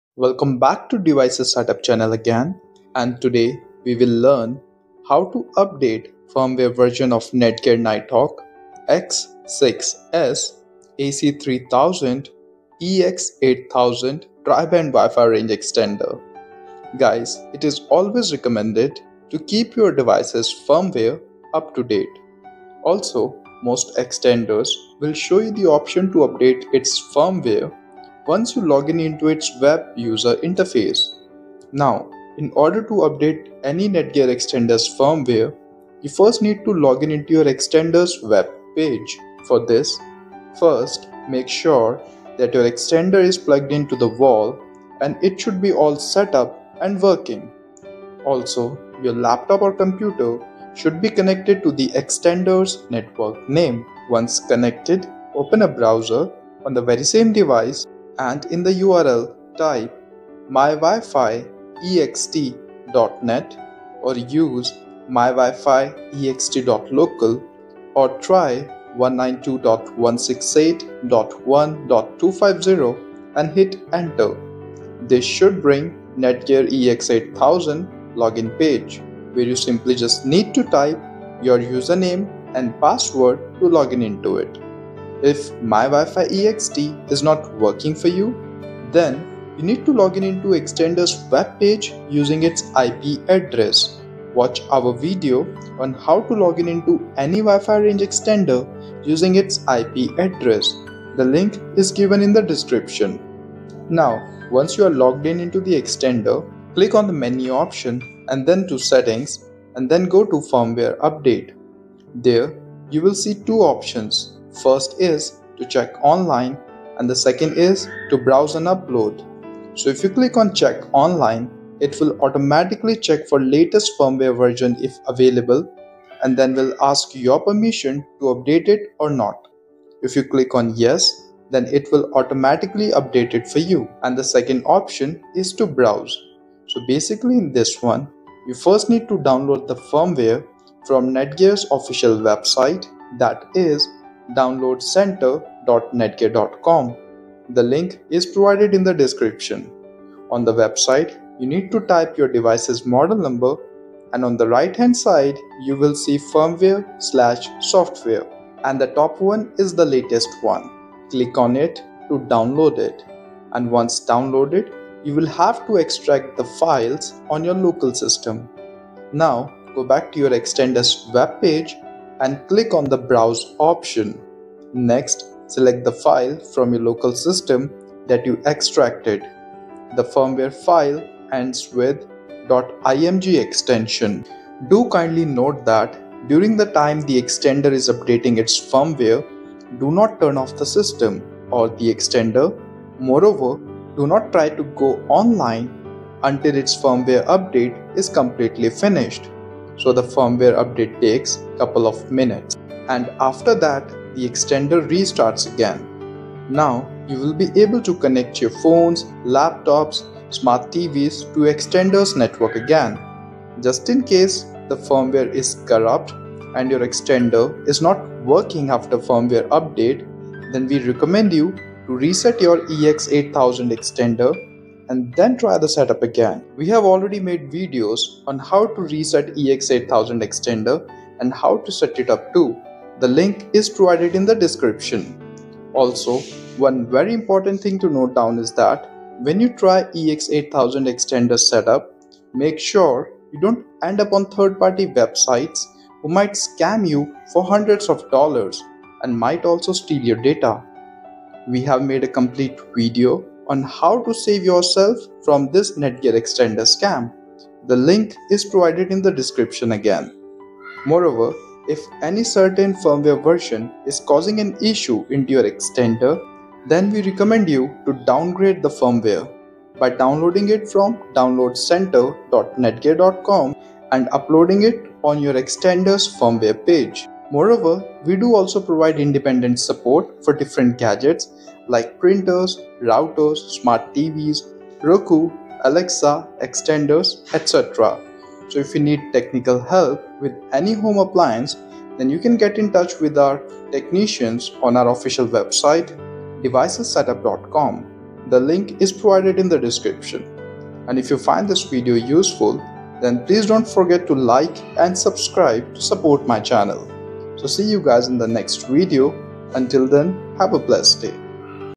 How to Update Netgear Ex8000 X6S Firmware Version? Netgear Extender Software Update Guide
Netgear ex8000 x6s ac3000 firmware update can be done by logging in into the extender web management page. Now if myifiext.net login is not working for you then you will have to login into the extender using its ip address. Watch our complete step by step guide here to update your netgear extender software.